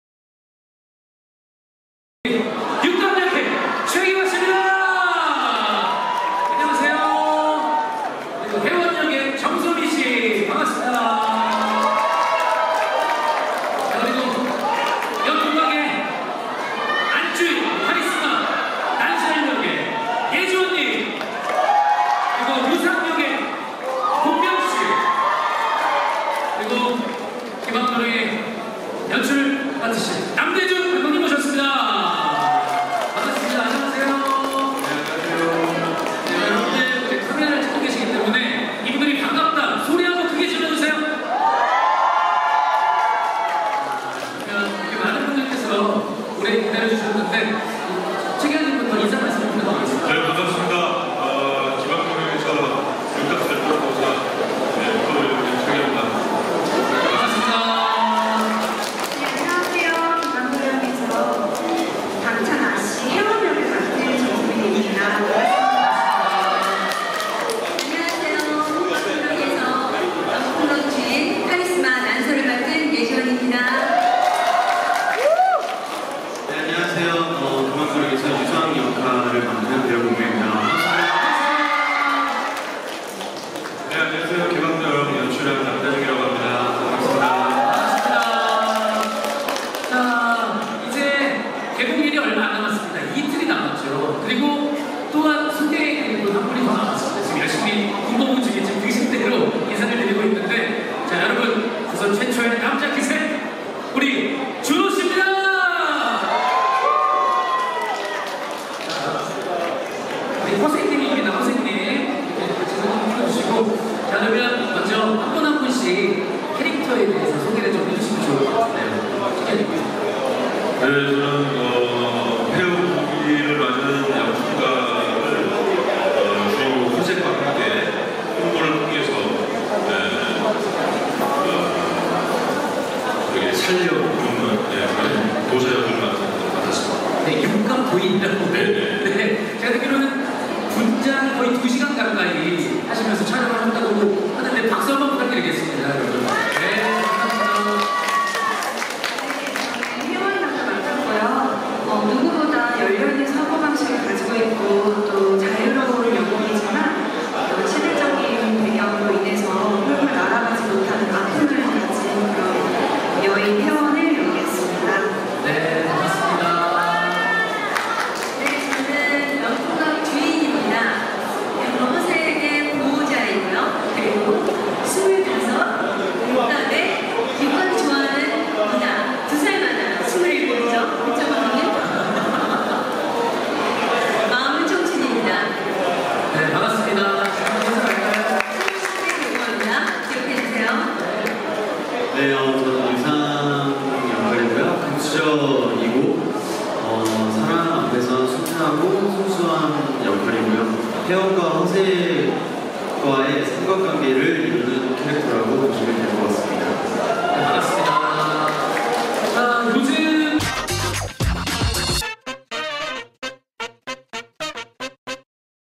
190708 공명X정소민X최귀화X예지원, 행사 시작 인사말 (영화 '기방도령' 레드카펫)
8일 오후 서울시 송파구 롯데시네마 월드타워에서 열린 영화 '기방도령' 레드카펫 행사가 열려 공명, 예지원, 정소민, 최귀화가 참석해 토크를 진행하고 있다.

영화 '기방도령'은 폐업 위기의 기방 '연풍각'을 살리기 위해 꽃도령 허색(준호)이 조선 최고의 남자 기생이 되어 벌이는 코믹 사극으로 오는 10일(수) 개봉한다.